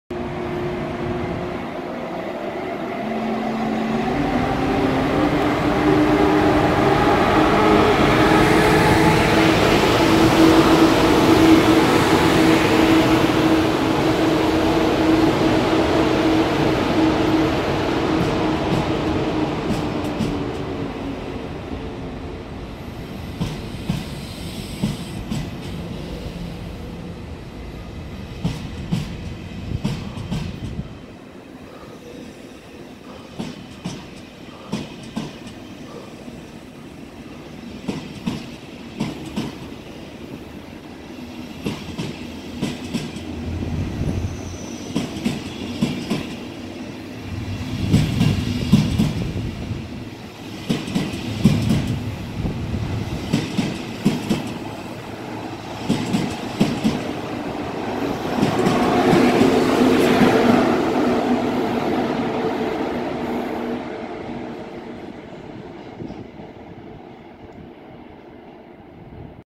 UltraHD 4K ICE 1 BR401 566-5 als ICE 789 Hamburg Altona-München HBF
🇺🇦Електропоїзд ICE 1 BR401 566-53 рейсом ICE 789 Гамбург-Мюнхен відправляється від станції Гамбург Головний
Станція Гамбург Головний ; Нім
ецька залізниця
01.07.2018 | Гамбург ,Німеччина

🇷🇺Електропоезд ICE 1 BR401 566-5 рейсом ICE 789 Гамбург-Мюнхен отправляется от станции Гамбург Главный
Станция Гамбург Главный ; Немецкая железная дорога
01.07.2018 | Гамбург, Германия

🇩🇪ICE 1 BR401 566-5 als ICE 789 Hamburg Altona-München HBF fährt ab Hamburg HBF
Hamburg Hauptbahnhof; DeutschBahn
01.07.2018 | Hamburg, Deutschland
Der Autor des Videos - wl/trains

🇬🇧ICE 1 BR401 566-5 trip ICE 789 Hamburg Altona-Munich HBF departing from Hamburg station
Hamburg Main Station; DeutschBahn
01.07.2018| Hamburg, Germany
The author of the video - wl/trains